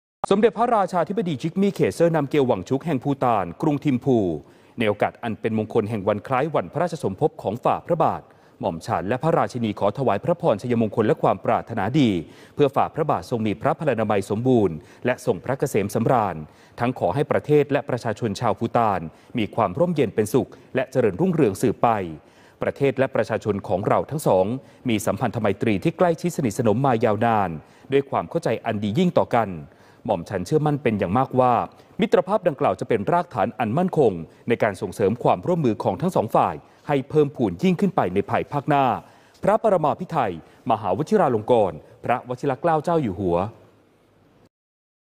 พระราชินี | พระราชสาส์น | สมเด็จพระราชาธิบดีแห่งภูฏาน | 21 ก.พ. 65
เมื่อวันที่ 21 กุมภาพันธ์ 2565 พระบาทสมเด็จพระเจ้าอยู่หัว ทรงพระกรุณาโปรดเกล้าฯ ให้ส่งข้อความพระราชสาส์นถวายพระพรชัยมงคลไปยัง สมเด็จพระราชาธิบดีแห่งภูฏาน ในโอกาสวันคล้ายวันพระราชสมภพของสมเด็จพระราชาธิบดีแห่งภูฏาน ซึ่งตรงกับวันที่ 21 กุมภาพันธ์ 2565 ความว่า สมเด็จพระราชาธิบดีจิกมี เคเซอร์ นัมเกล วังชุก แห่งภูฏาน กรุงทิมพู ในโอกาสอันเป็นมงคลแห่งวันคล้ายวันพระราชสมภพของฝ่าพระบาท หม่อมฉันและพระราชินีขอถวายพระพรชัยมงคลและความปรารถนาดี เพื่อฝ่าพระบาททรงมีพระพลานามัยสมบูรณ์และทรงพระเกษมสำราญ ทั้งขอให้ประเทศและประชาชนชาวภูฏานมีความร่มเย็นเป็นสุขและเจริญรุ่งเรืองสืบไป